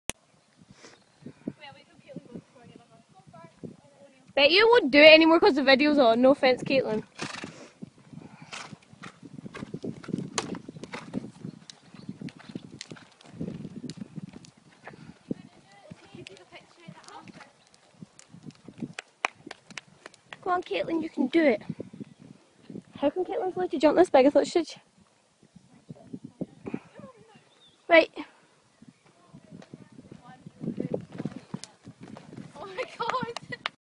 1.10m on Watson:)
and jess doing it on hankeypank lol
and lucy's encouragement was fabulous